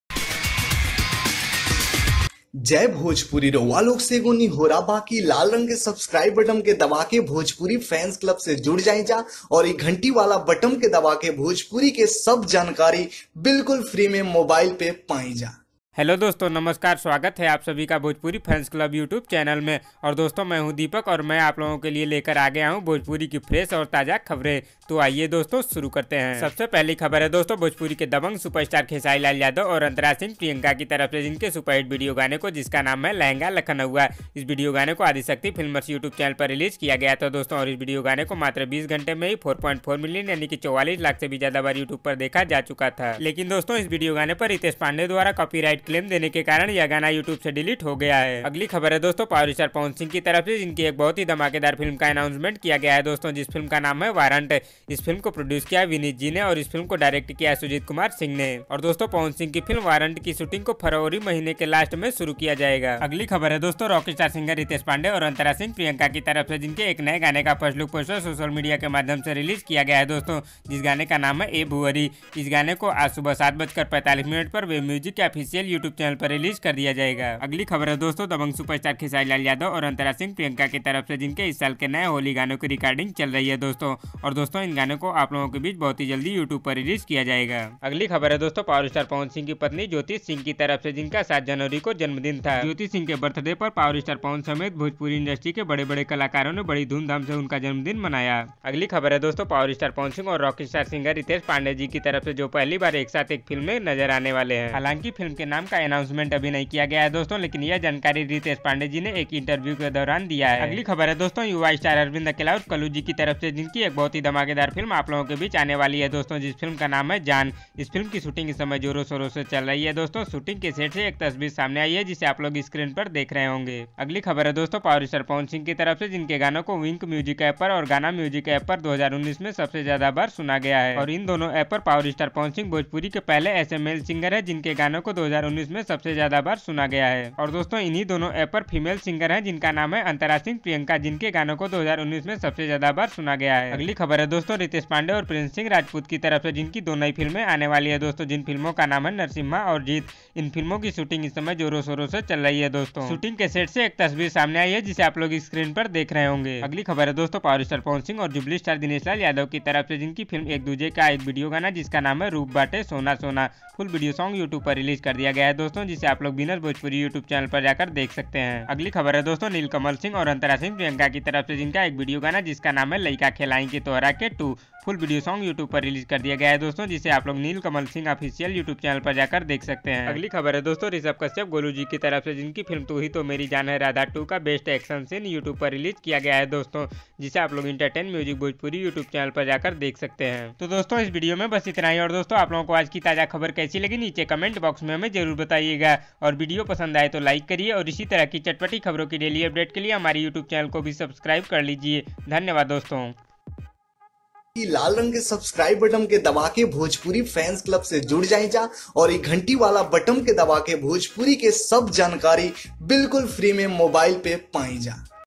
आज के ताज़ा खबर || #08-01-2020 || रितेश ने करवाया खेसारी का गाना डिलीट, पवन सिंह ने दिया साथ
Aaj Ke Taja Khabar - 08/01/2020 - Pawan Singh, Khesari Lal Yadav, Akshara Singh, Nirahua, Kajal Raghwani, Amrapali Dubey, Ritesh Pandey, Samar Singh, Pramod Premi Yadav - Bhojpuri News 2020

Report By - Deepak